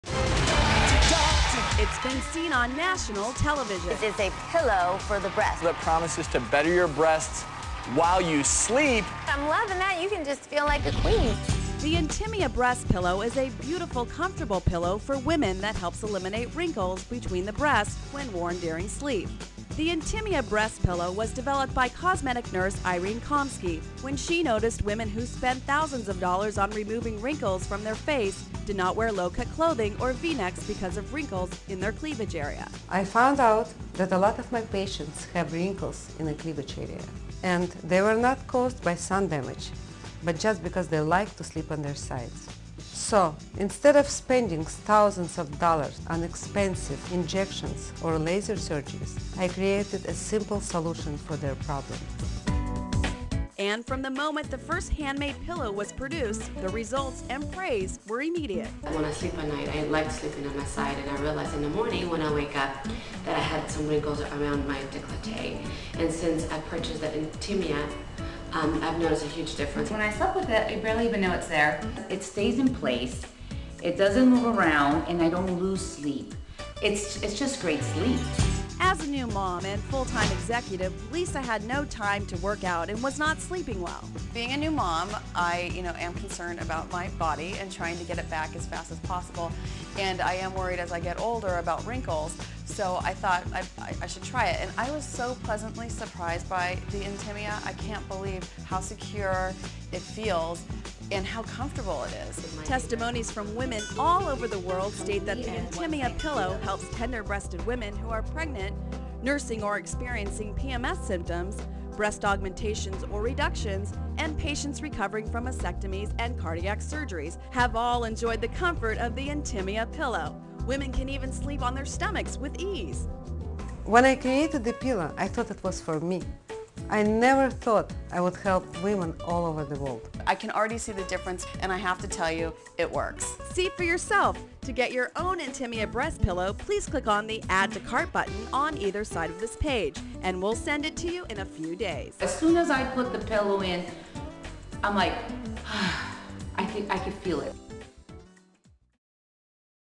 How to treat your chest wrinkles with the Breast Pillow.
Intimia® Breast Pillow is a unique specialty bra for women designed to provide improved sleep comfort and a chest wrinkles treatment for women of all ages. Intimia® Breast Pillow comfortably separates the breasts without restricting the blood flow. It provides excellent breast support and can be used by women while breastfeeding or by women recovering after breast augmentation or heart related surgeries.